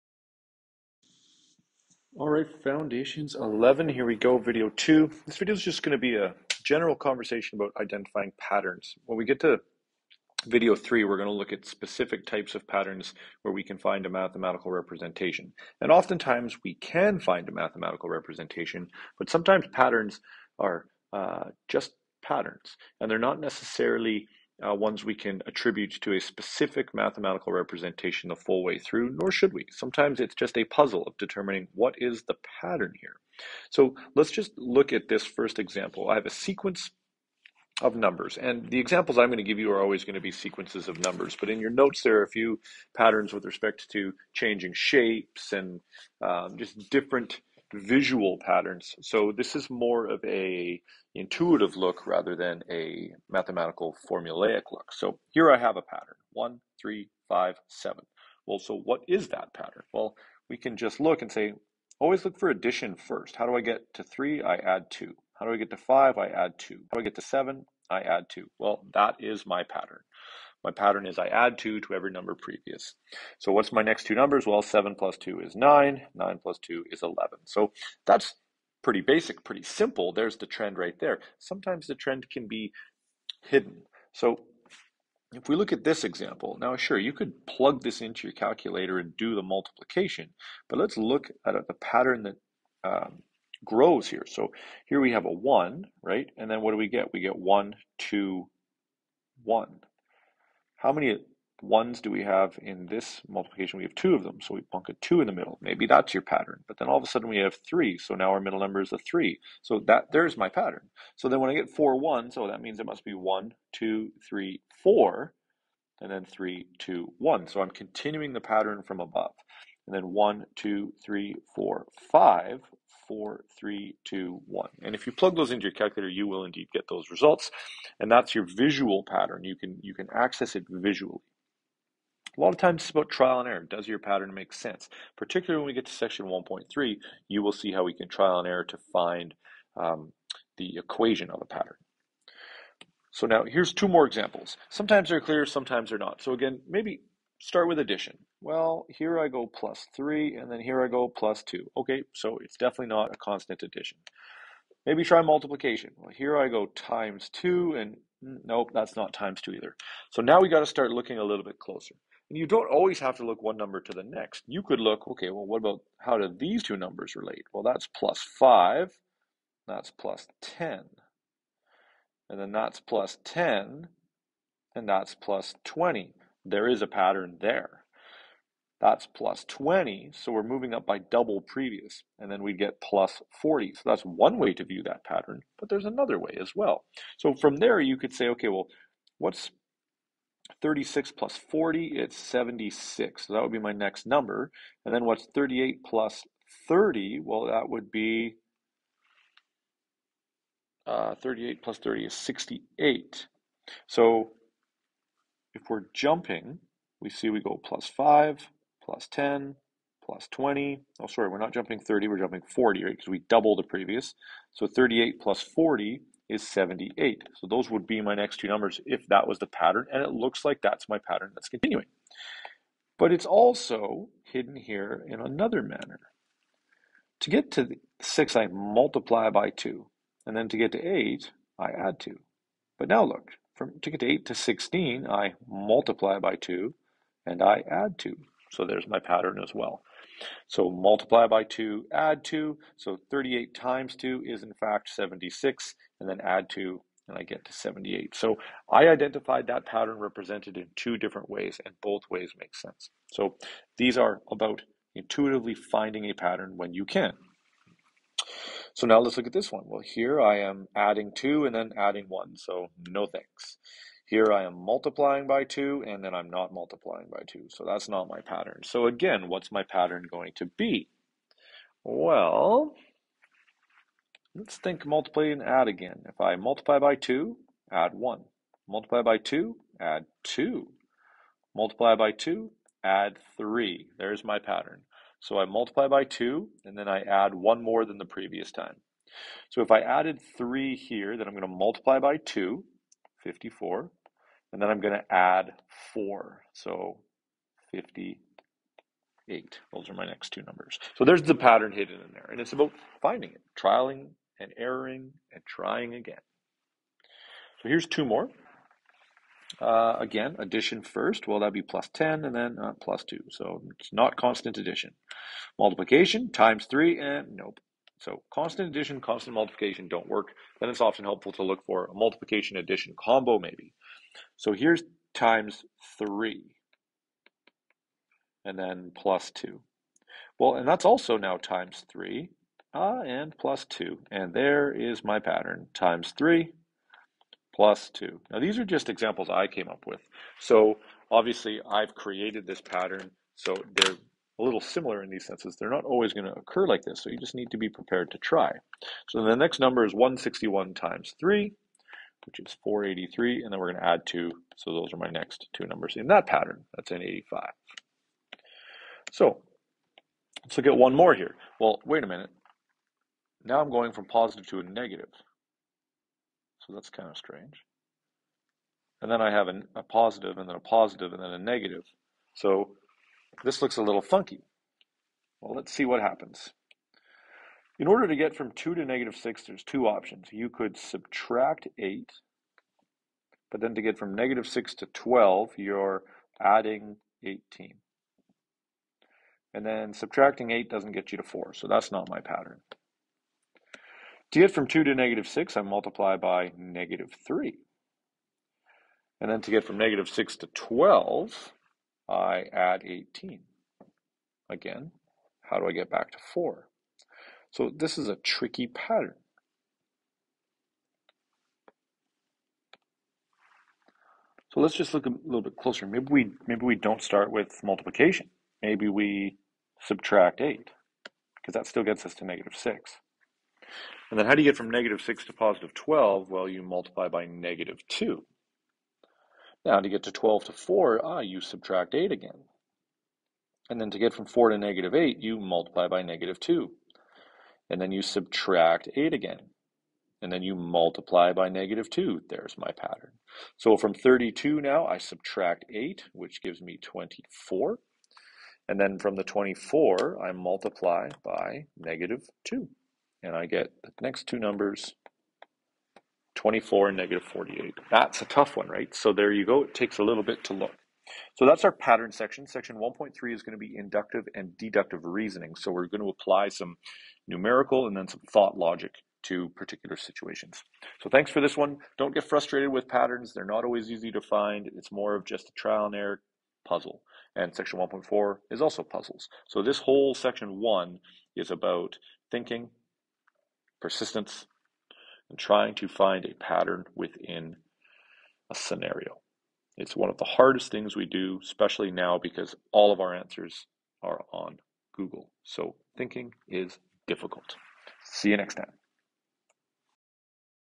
Foundations of Math 11 - Video #2: Patterns
Some patterns are easy to distinguish, some not so much. We can apply mathematical equations to many patterns, but here we use observation and intuition only.

Good Luck!!